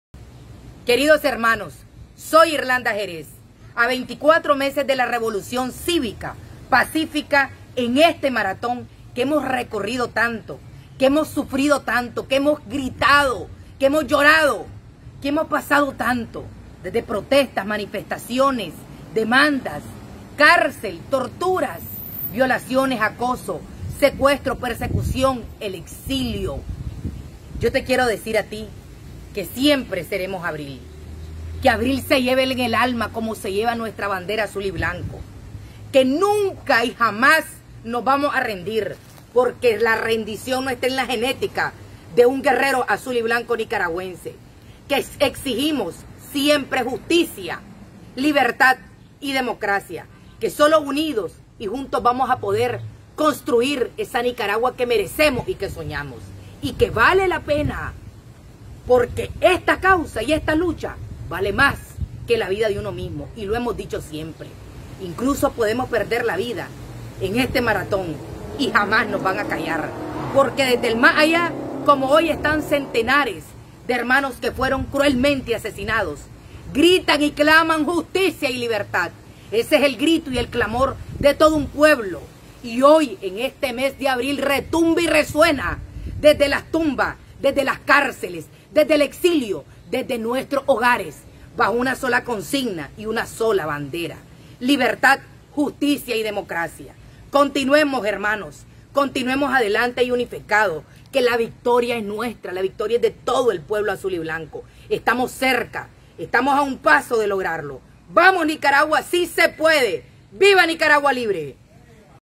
La Victoria se Acerca estamos de 1 Paso de Derrocar a Ortega/Murillo//Irlanda Jerez Expresa
🔊🔊🔊SE SUGIERE ALTA DISCRESION CON LA NOTICIA APTA PARA MAYORES DE +18
Irlanda Jerez Expresa,Denuncia,Dice
Líder Nacional Opositora y Ex Carcelada Politica Nicaragua🔊🔊🔊

A 24 meses de la Revolución Cívica el clamor de todos los nicaragüenses sigue siendo Justicia, Libertad y Democracia

Abril lo llevamos en el Alma
La lucha sigue!!!

Todos Somos UNO!!!
Unidos INVENCIBLES!!!
Nicaragua será REPÚBLICA!!!